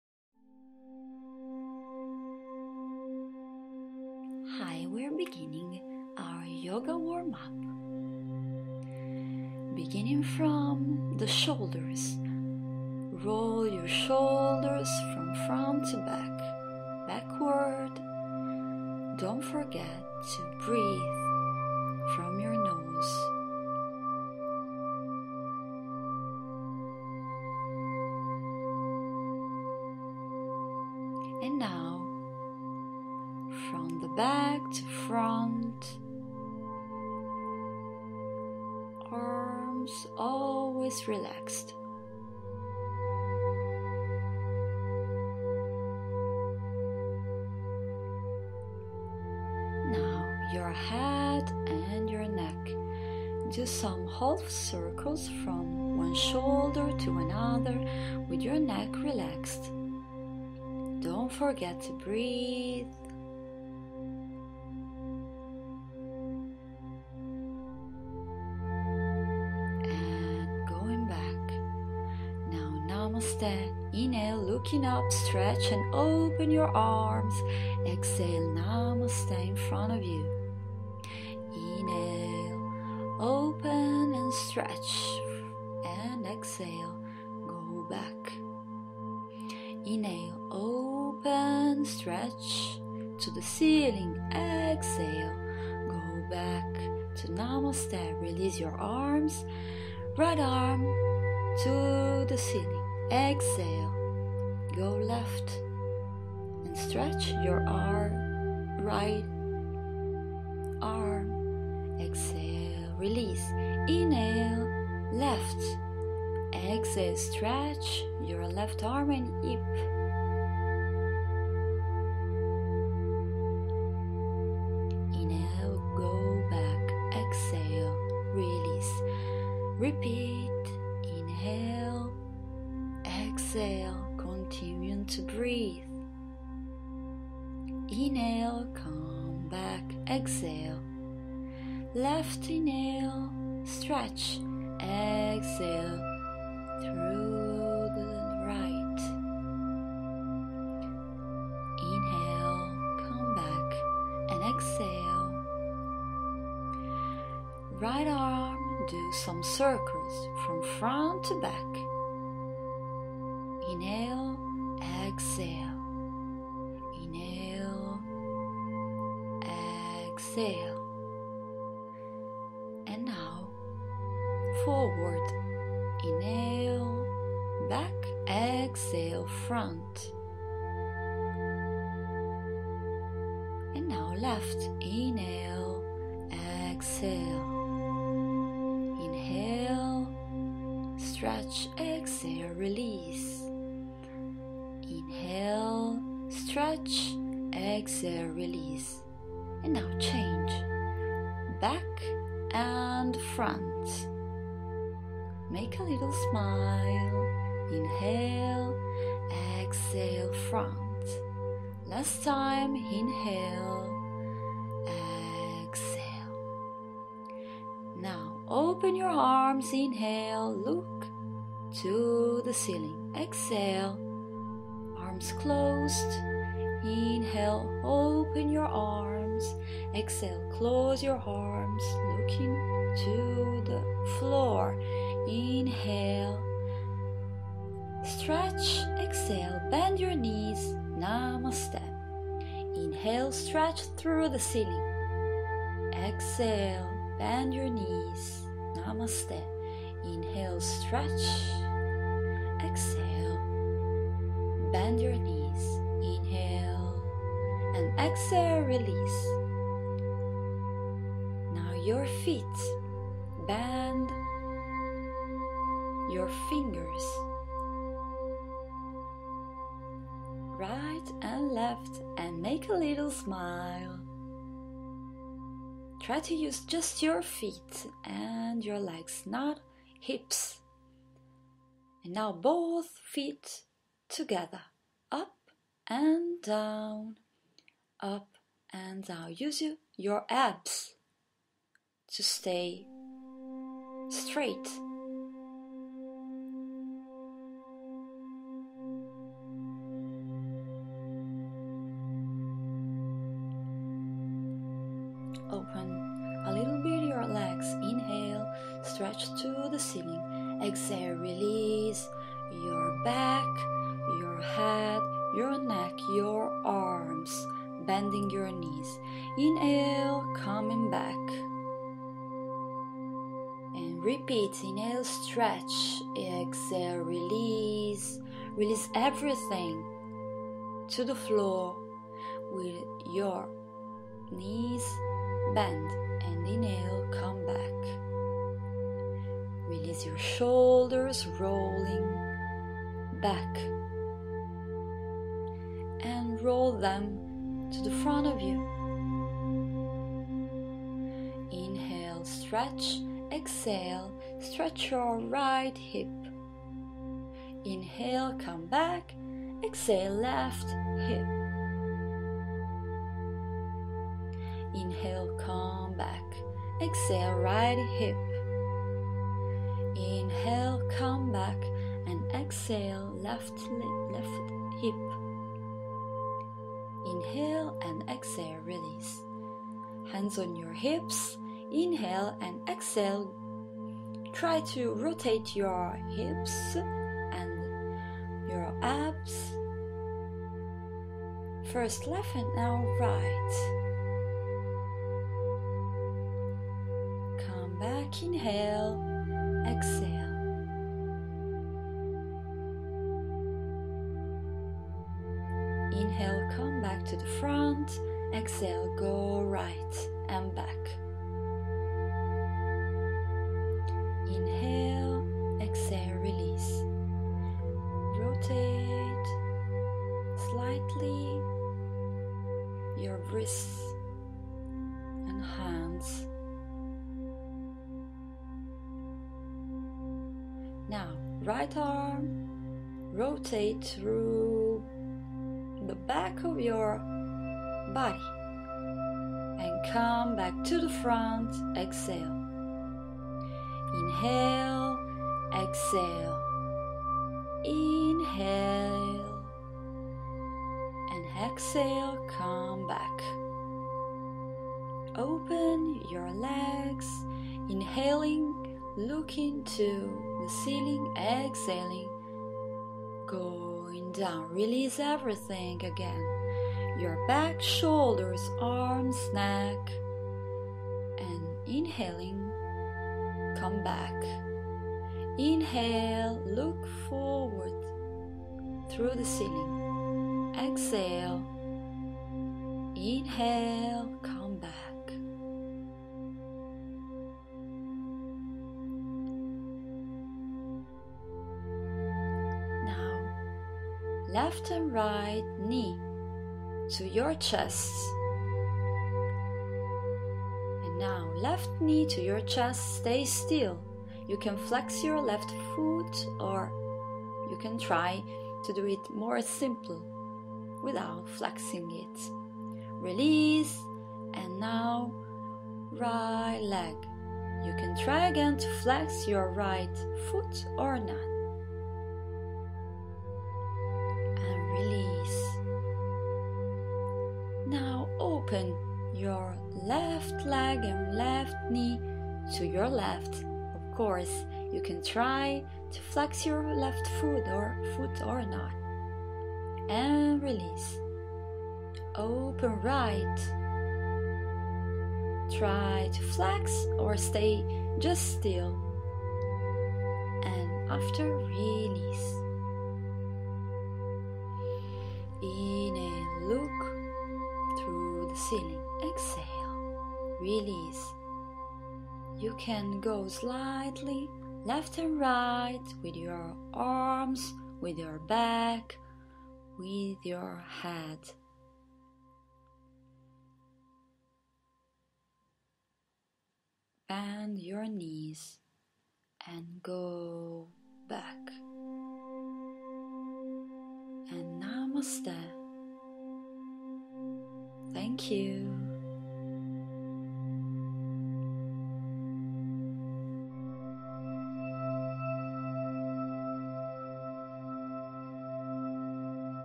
Yoga Warm Up
Get ready with me and do a little warm up for your yoga classes and for your other kind of workout!